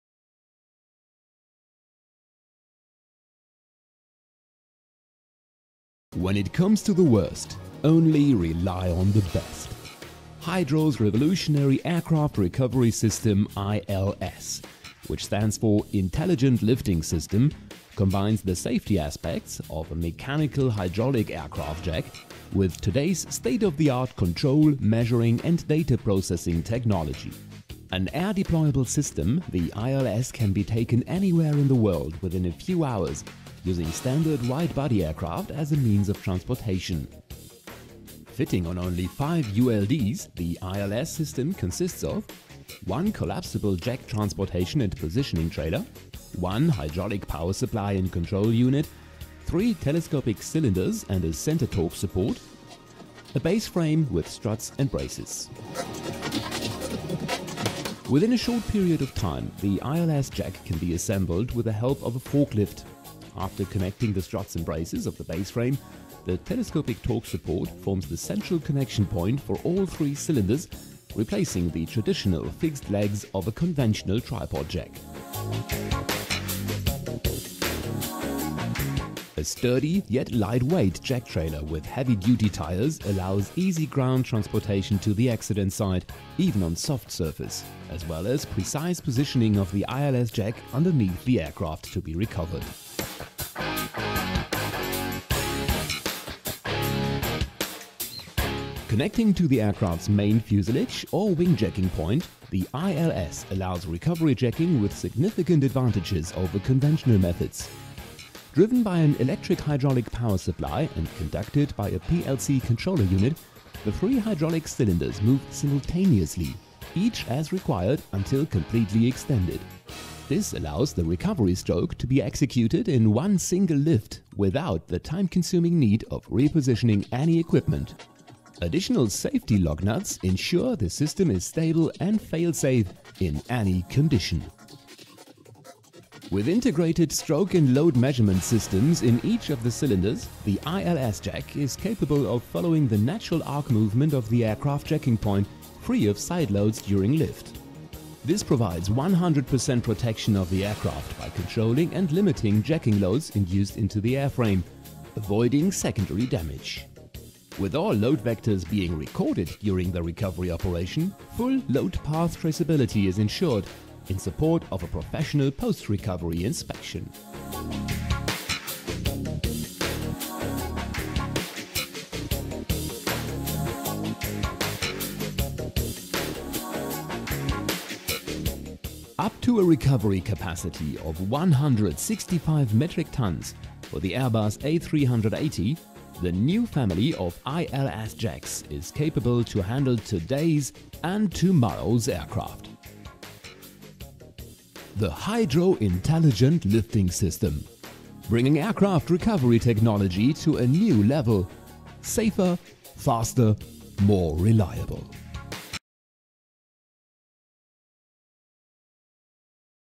HYDRO Systems KG - ILS Recovery Jacking
Innovative aircraft recovery jack for Airbus A380.